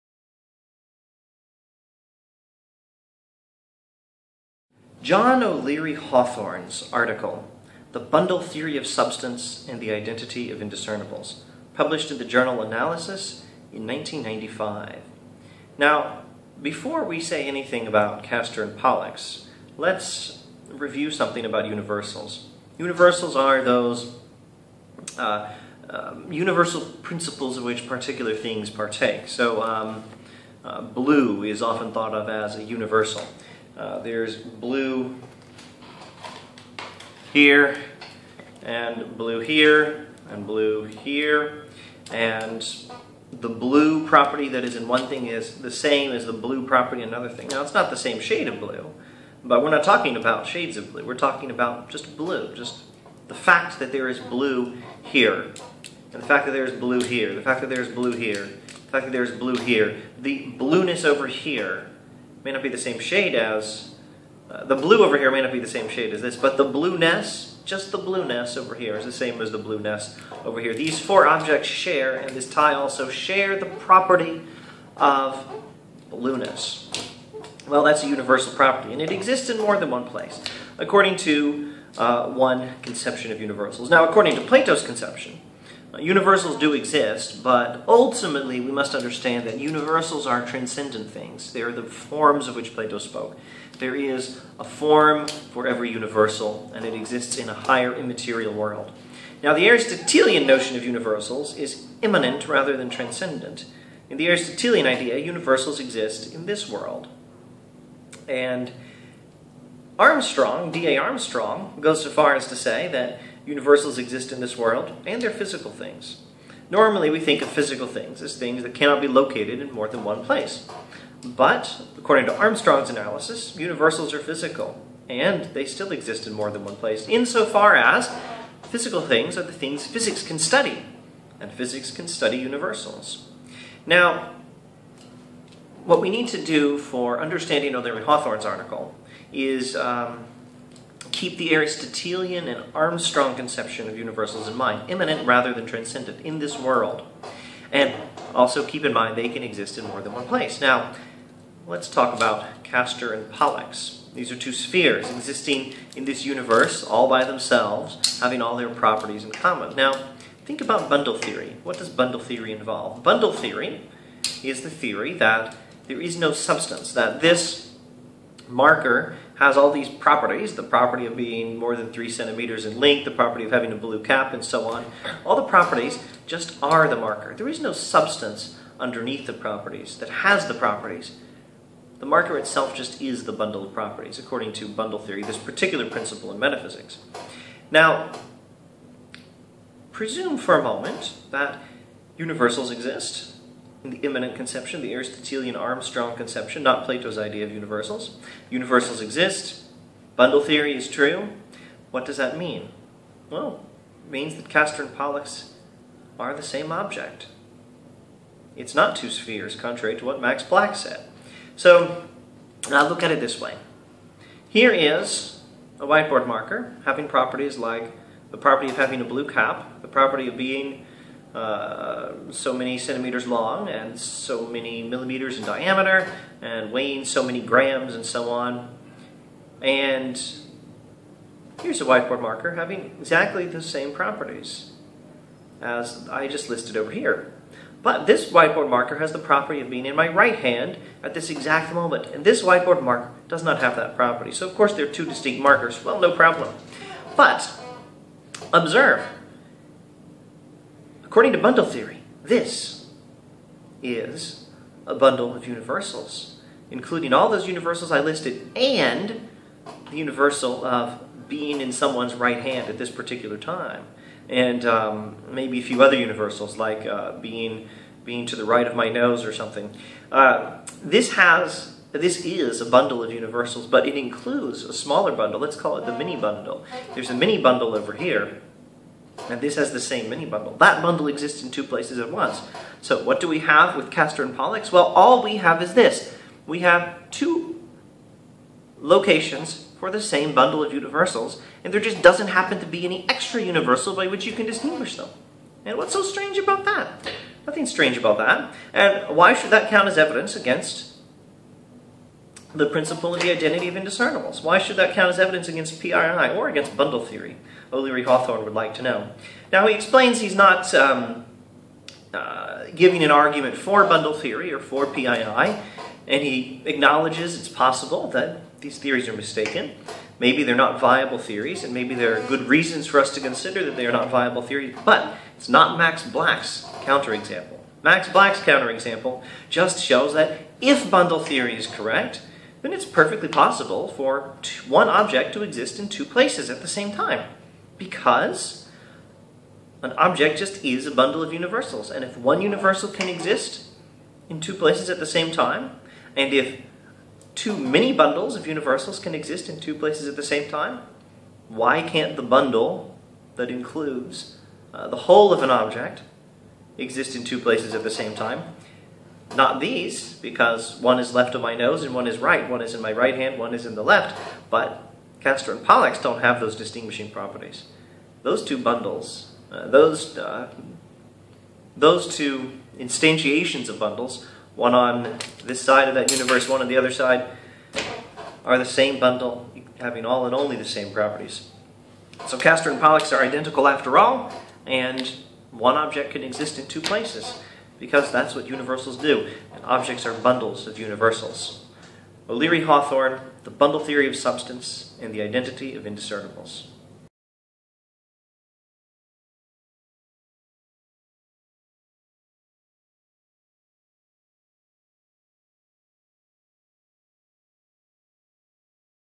John O’Leary-Hawthorne's “The Bundle Theory of Substance and the Identity of Indiscernibles"
Bundles of universals already exist in two places.  If one of them happens to be a sphere named Castor in one place and Pollux in another--so what?